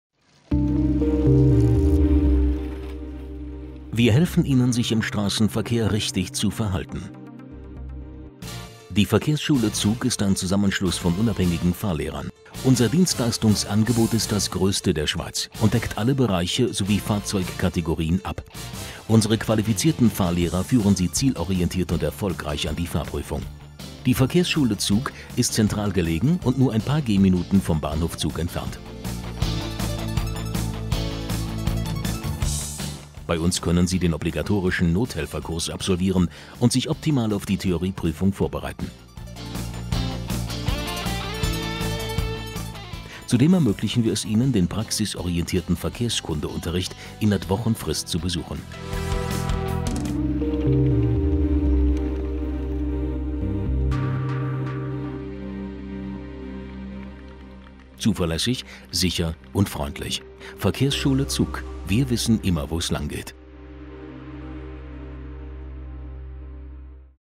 Verkehrsschule Zug, Zug; Sicherheit durch Erfahrung: Commercials / Promotional: SCHWEIZ: by ...
Verkehrsschule Zug: Baarerstrasse 19, 6300 Zug, SCHWEIZ Tel: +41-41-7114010 Email: Die Verkehrsschule Zug ist ein Zusammenschluss unabhängiger Fahrlehrerinnen und Fahrlehrer, die gemeinsam seit über dreissig Jahren eine optimale Ausbildung im Strassenverkehr anbieten.